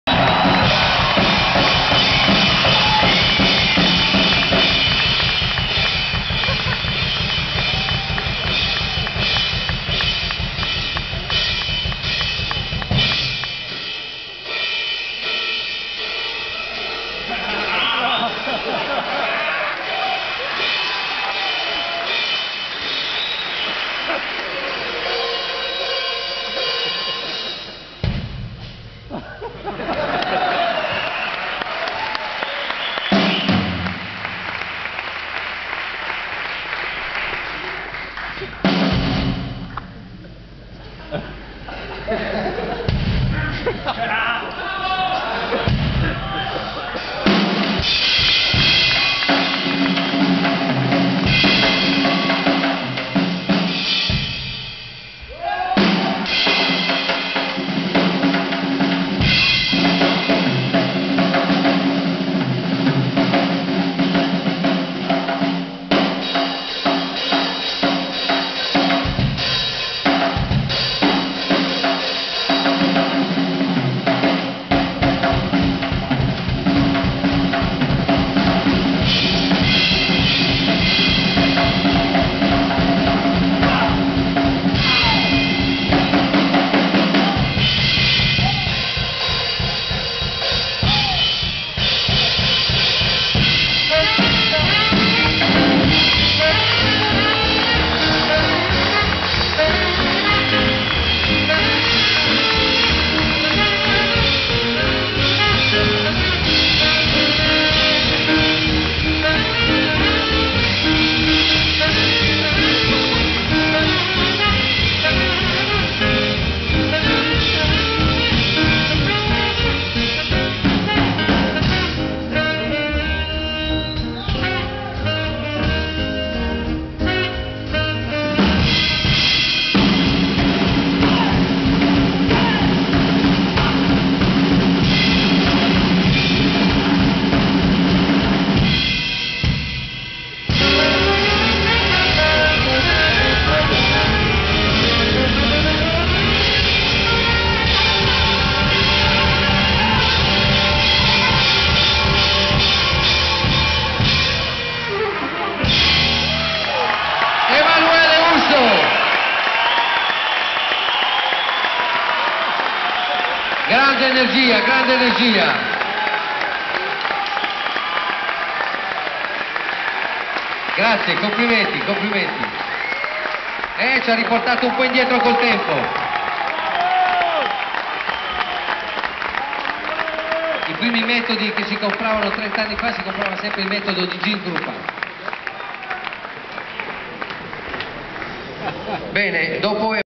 La lunga notte della Batteria - 28 Maggio 2009 - Video 1
Esibizione di Emanuele Urso: The Amazing Drummer!!